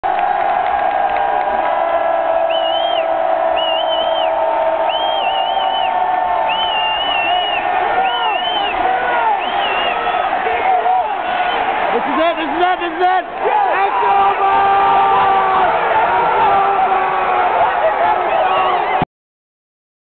Texas Longhorns Beat USC and Storm Field
This is the last play of the 2006 Rose Bowl. With 8 seconds left, USC is down 41-38 and is about 10 yards short of reasonable field goal range. As Matt Leinert rolls to his left, you can hear me say, "This is it, this is it, this is it!" as I had one eye on the clock and one eye on the play and could tell that with Leinert wasting time scrambling around that USC would not get off another play. I then screamed, "it's over!" repeatedly as the Horns stormed the field